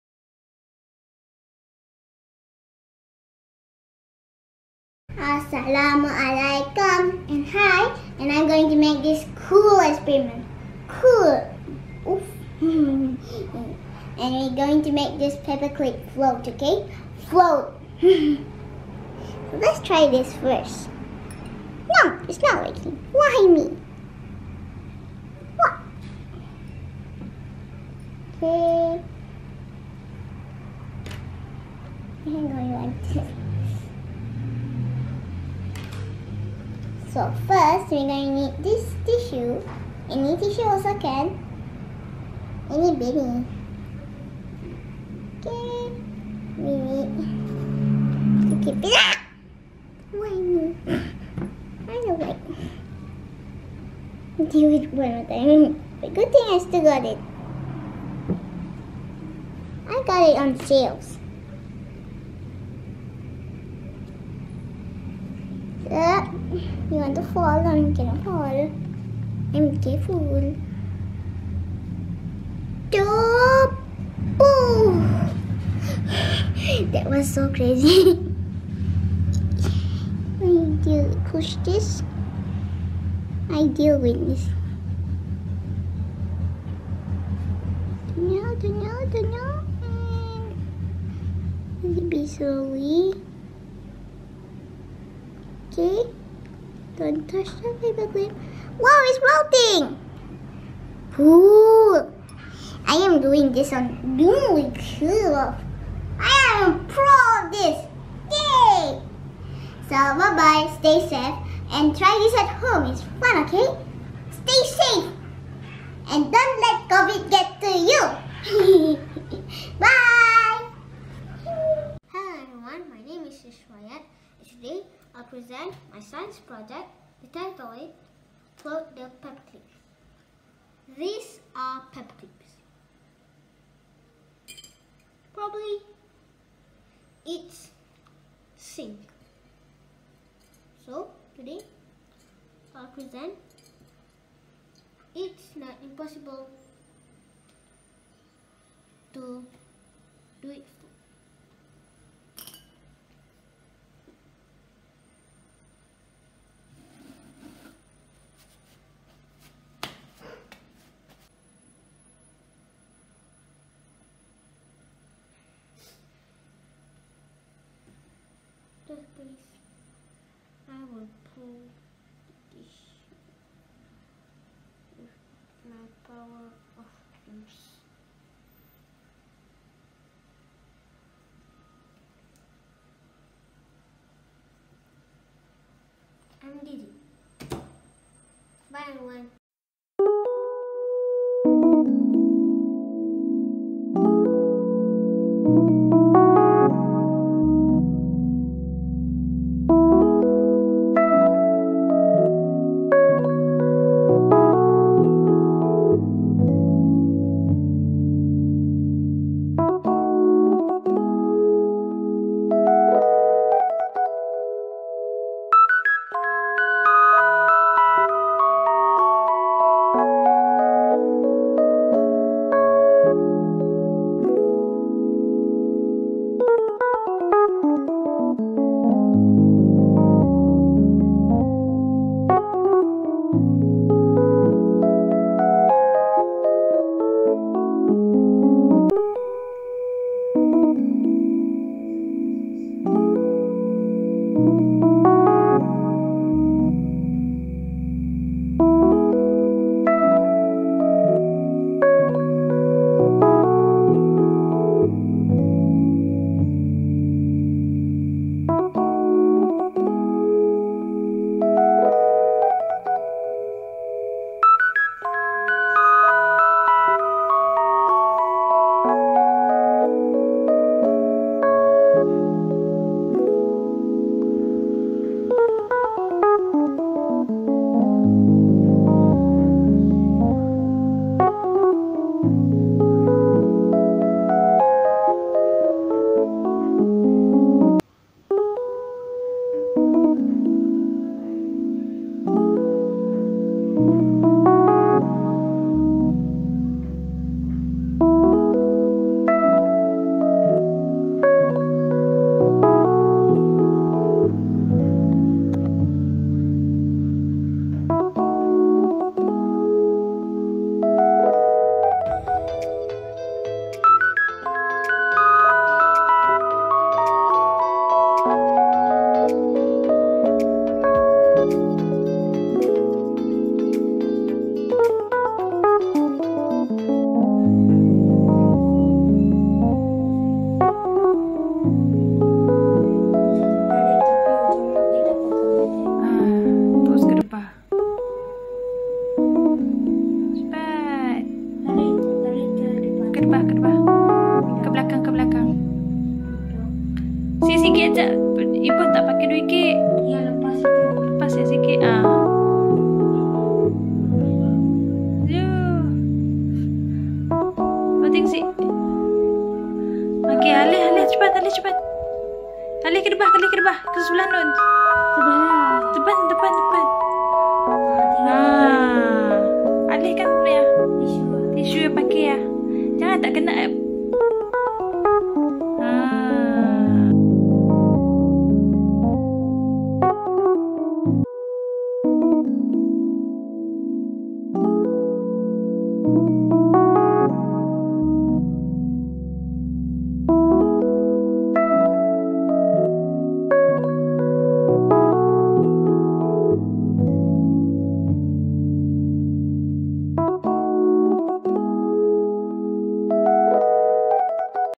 Floating Paperclips Challenge 2
Koleksi oleh murid-murid 2 Bestari:
1. Nurul Saida
2. Rizq Fayyadh
3. Arianna Zara
4. Hizqeel Farhan
5. Mohd Harith
6. Zharif Imtiyaz
7. Awang Affan
8. Aneeq Khalis
9. Abang Aidh Ridhwan (and brother)
10. Qayser Dasuqi

Aktiviti ini merupakan sebahagian PdPc sepanjang PKP/ PKPP akibat wabak pandemik Covid-19 melanda negara kita.

Murid-murid telah diberikan tugasan menyiapkan challenge ini dan berkongsi hasilnya melalui Google Classroom. Hasil dan komitmen yang ditunjukkan murid-murid sangat memuaskan.

Tahniah anak-anak dan terima kasih kerana sentiasa positif dalam mengikuti pembelajaran Sains.